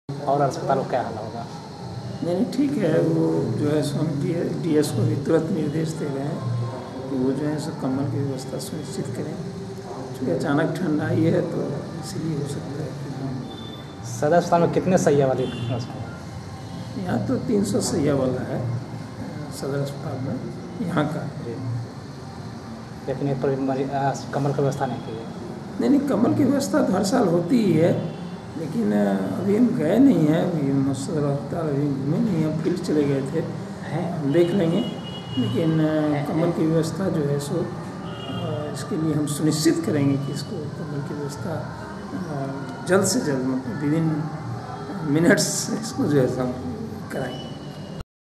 PNA NEWS AGENCY
From --- Nalanda --- जी हाँ अगर आप मरीज हैं और बीमार अवस्था में ठंड के मौसम में सदर अस्पताल बिहारशरीफ में भर्ती होने के लिए जा रहे हैं तो हो जाईये सावधान क्यो की आपको कङाके की ठंड से बचने के लिए अपने घरो से कंवल लेकर जाना होगा । जी हाँ यह हम नहीं बल्कि मीडिया की कैमरे में कैद तस्वीरे वयां कर रही है यह हाल बिहार में बिकास का दावा करने वाले मुख्यमंत्री नीतीश कुमार के गृह जिला नालंदा की है जहाँ बिकास कार्यो की पोल खुलती दिख रही है,| Byte --- डॉ सुबोध कुमार सिंह सिविल सर्जन बिहारशरीफ सदर अस्पताल |